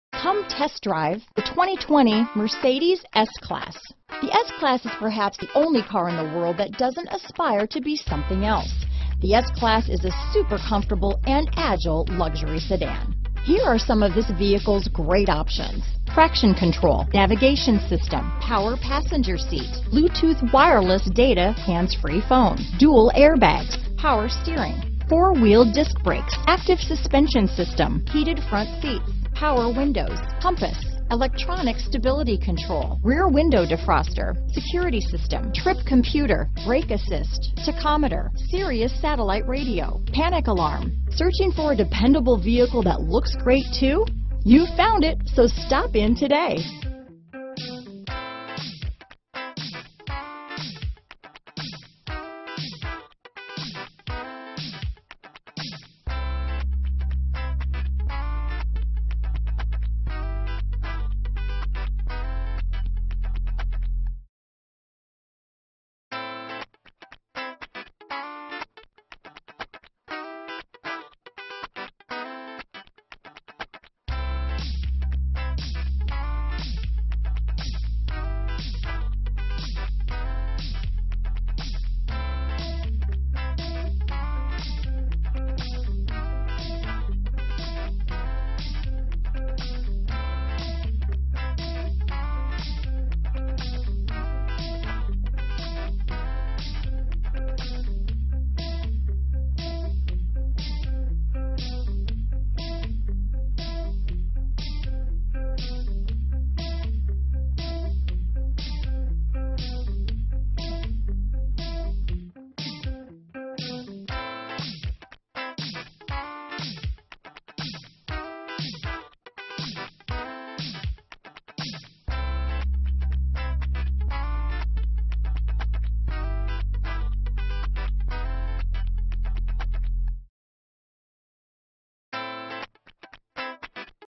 2020 Mercedes-Benz S-Class Bonita Springs, Naples, FL 517779
Black New 2020 Mercedes-Benz S-Class available in Naples Florida at Mercedes Benz of Bonita Springs.
2020 Mercedes-Benz S-Class S 560 - Stock#: 517779 - VIN#: WDDUG8DB4LA517779

Mercedes Benz of Bonita Springs
14610 Tamiami Trail North